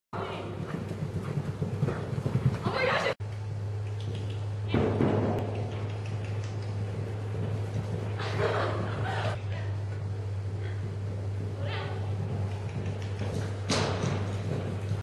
Team jumping with Peanut and Lance.
Leanne and I team jumping with Peanut butter and Lance! (: (:
First attempts for both of us ever. It's actually super fun.